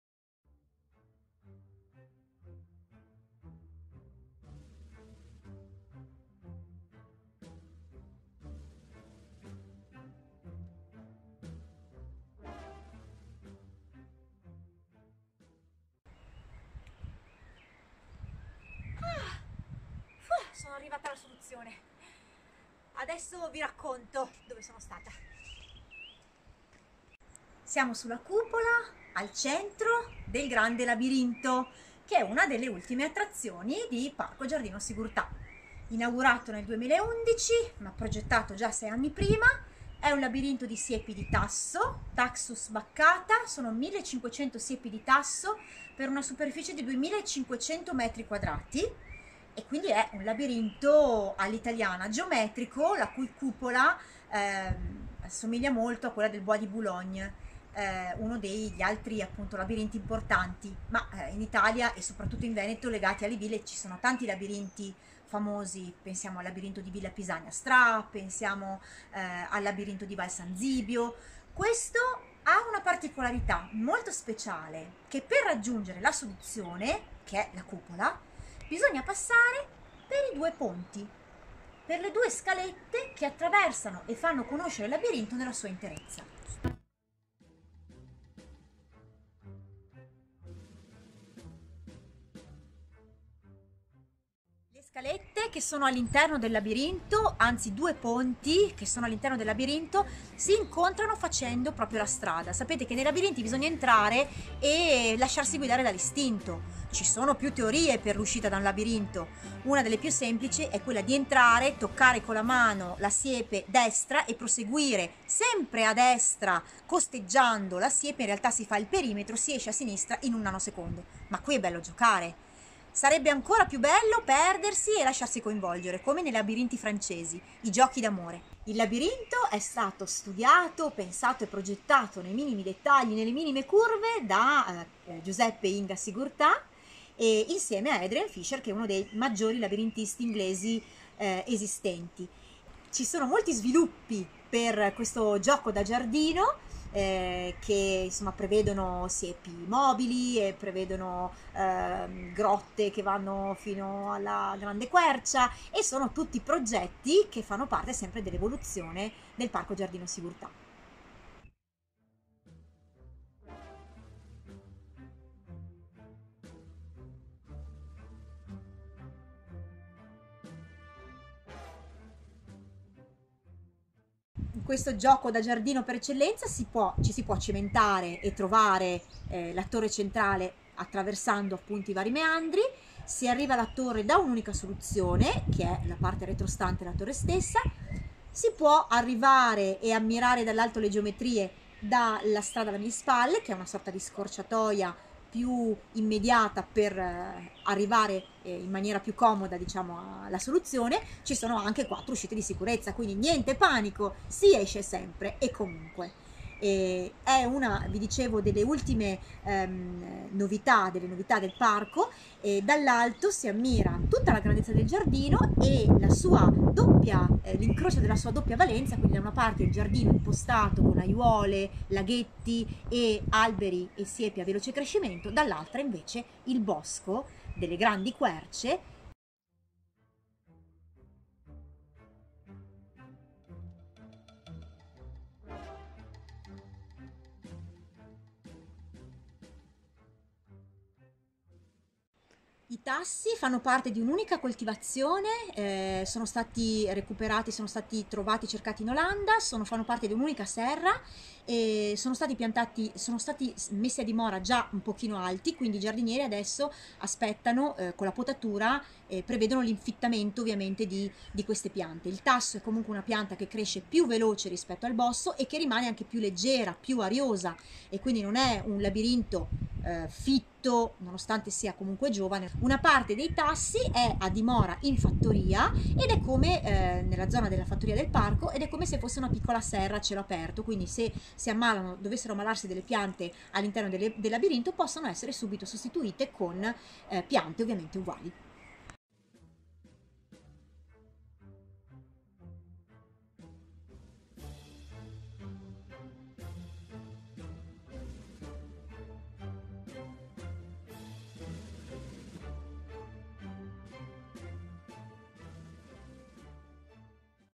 Il Parco in pillole: il Labirinto
IL PARCO IN PILLOLE

Al centro del dedalo sorge una torre, ispirata a quella del parco di Bois de Boulogne di Parigi, che presenta una cupola e due scale contrapposte (che raggiungono l'altezza di 2,50 metri): per i visitatori giungere alla torre sarà una vera e propria ricompensa della soluzione, poiché dall'alto si potranno ammirare le geometrie del percorso stesso e le altre attrazioni naturali del Giardino. Alice, guida del Parco da oltre 10 anni, ci racconta dei esemplari di Tasso (Taxus baccata L.) e del percorso che si snoda tra piante alte più di due metri e che si estende su una superficie rettangolare di 2500 metri quadrati.